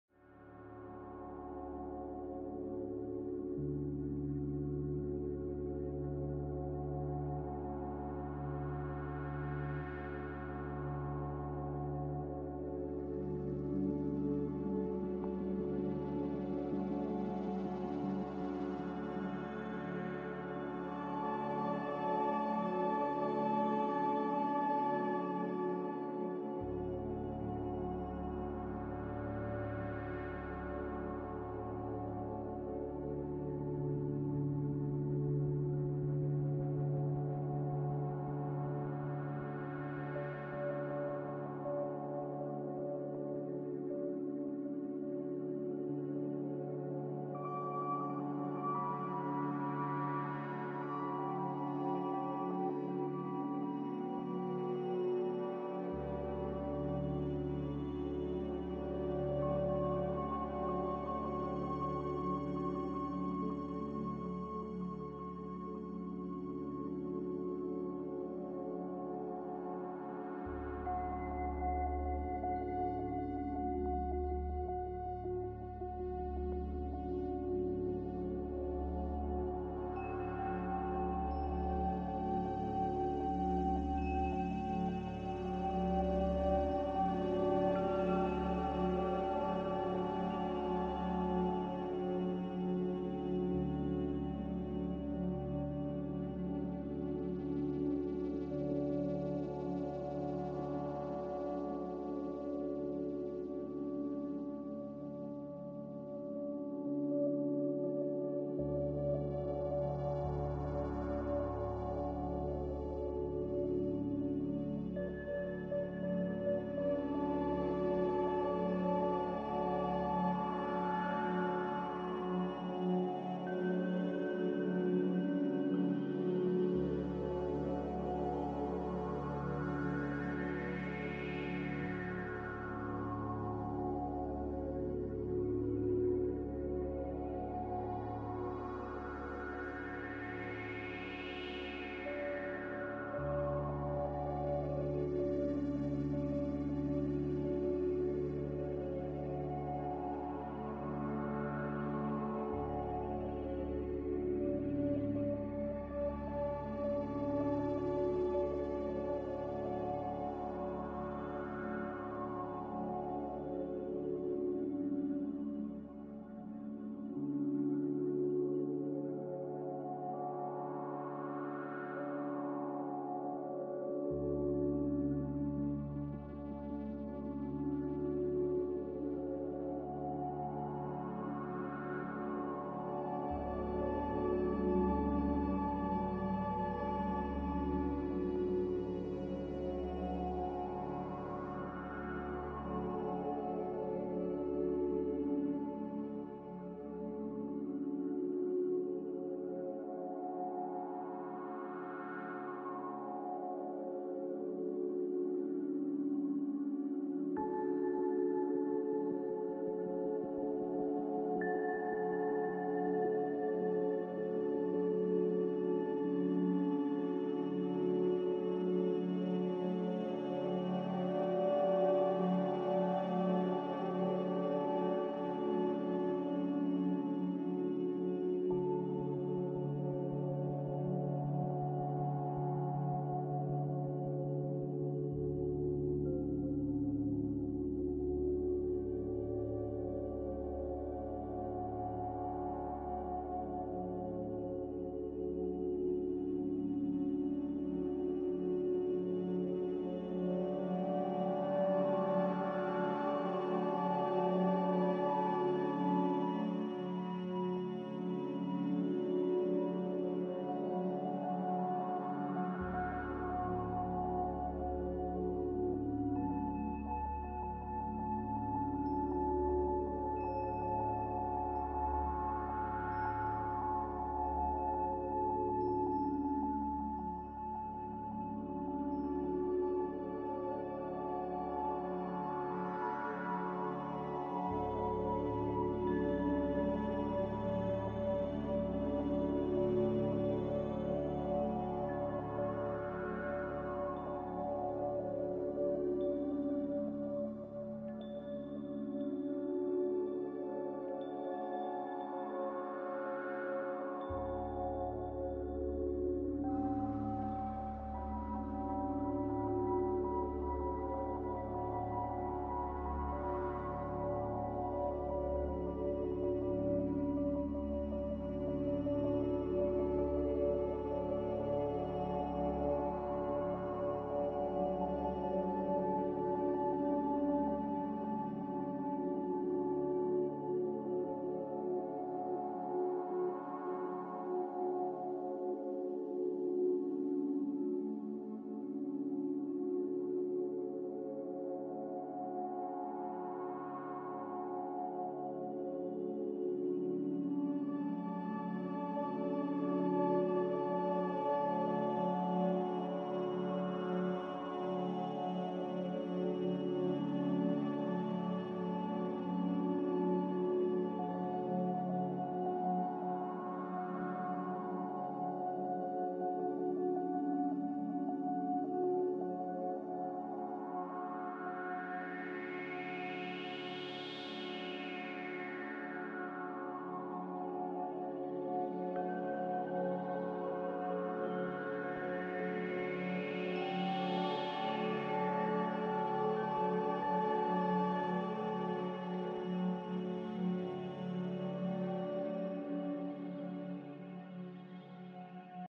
Unwind and Relax: 4K UHD Visuals Accompanied by Soothing Music - 4K Video
Want to take a break and relax? This video is a great way to wind down after a long day and relax in some calming 4K visuals.
Whether you're a student studying for exams or an office worker who wants to relax after a long day, this video is a great way to relax your mind and body. With soothing music and beautiful 4K visuals, this video is sure to help you unwind and relax!